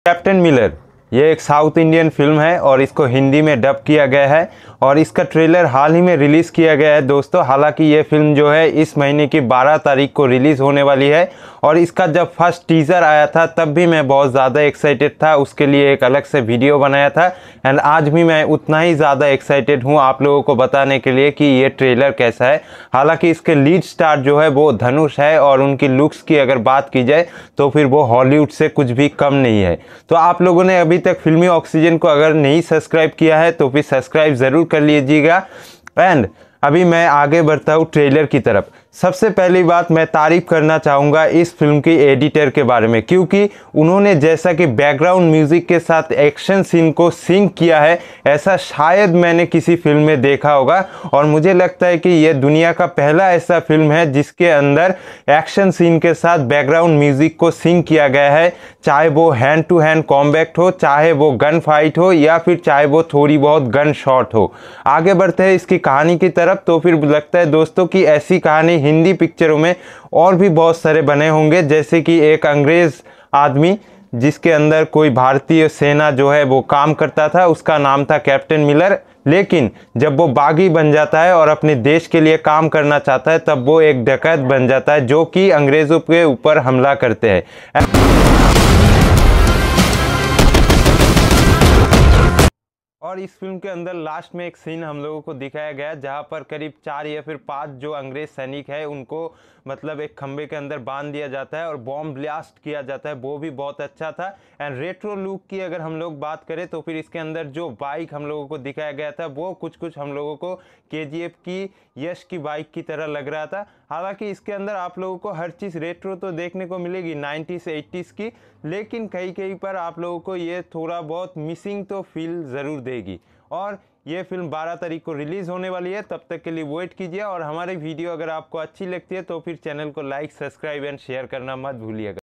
Captain Miller, the new action film of South, review by FILMY_OXYGEN
Captain Miller
Dhanush
South Indian film
Captain Miller official trailer review by filmy oxygen, Starr with dhanush, this is an Tamil action , advanture drama film , the film released in 12 Jan 2024, also dubbed in Hindi language, 2024 new South Indian film, new pan India film, 2024 new movie, review by filmy oxygen.